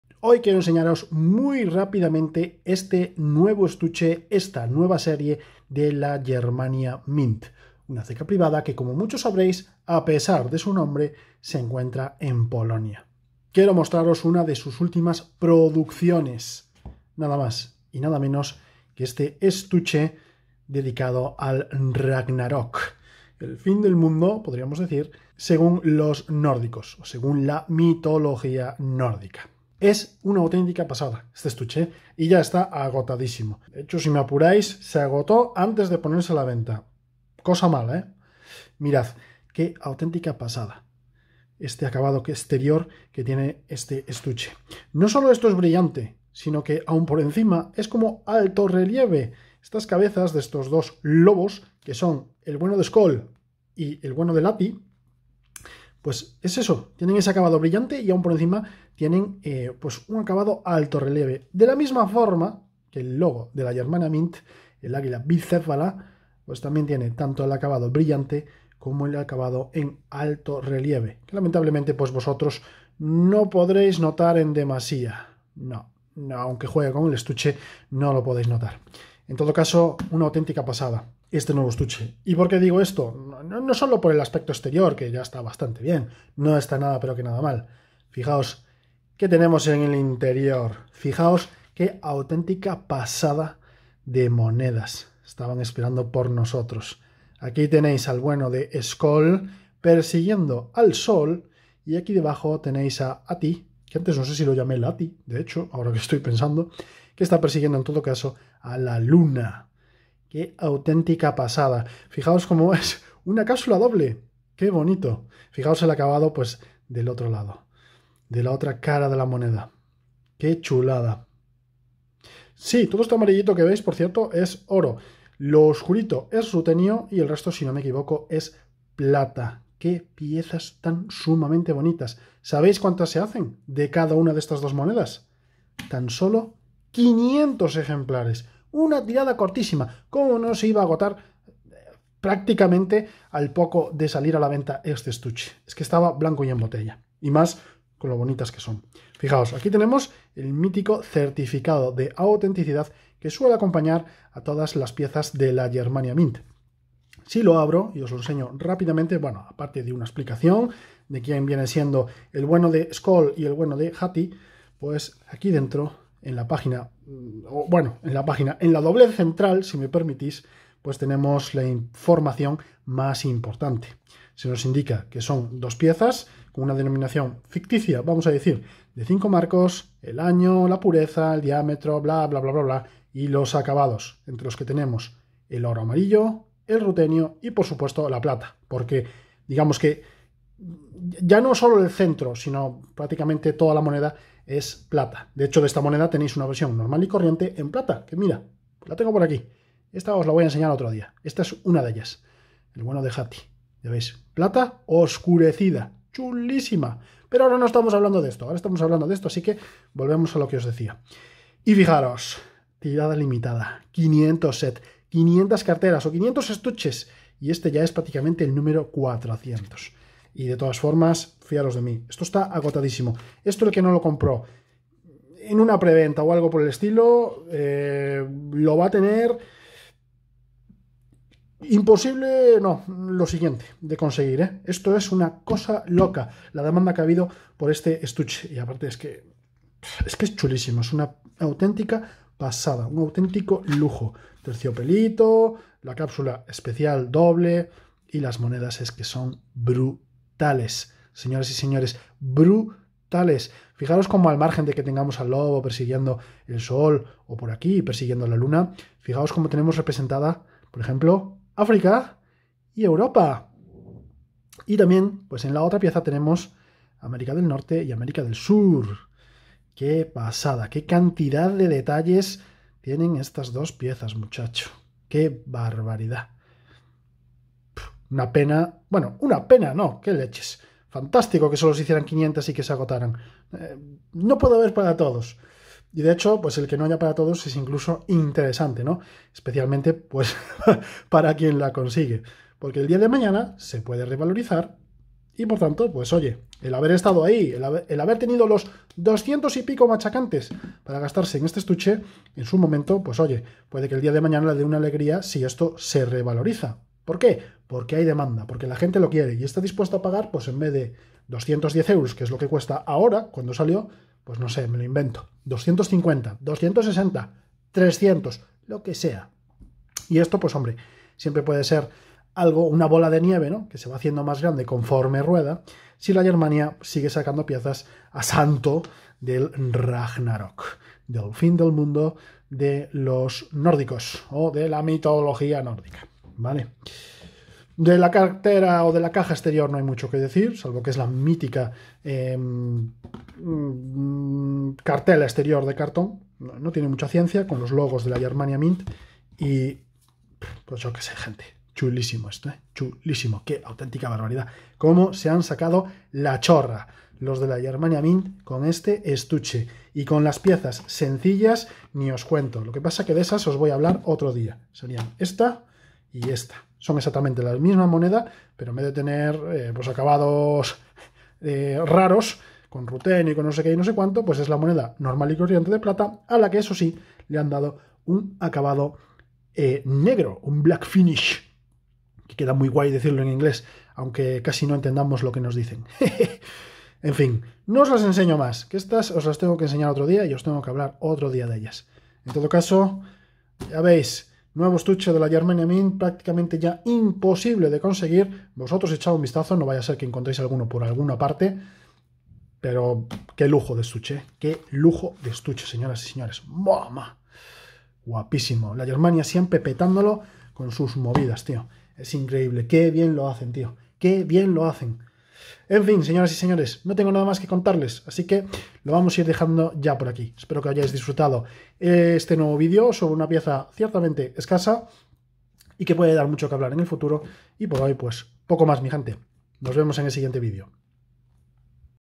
⚠️ SÓLO 500 MONEDAS PARA TODO EL MUNDO! | Germania Ragnarök
La Germania Mint se ha vuelto a superar con su nueva emisión dedicada al fin del mundo según la mitología nórdica: el Ragnarök.

En el estuche de la primera emisión de esta serie, nos podremos encontrar a los lobos Sköll y Hati persiguiendo al sol y a la luna... objetivo que consiguen precisamente al llegar el Ragnarök. Sólo se han fabricado 500 unidades de estos estuches y ha sido un éxito de ventas, se ha agotado en prácticamente todos lados. Seguramente ha ayudado el oro, rutenio y plata que toman parte en los excelentes acabados de estas dos monedas...

Por mis manos ha pasado uno, el número 396 y al que he podido dedicarle un vídeo rápido. Disfrutadlo.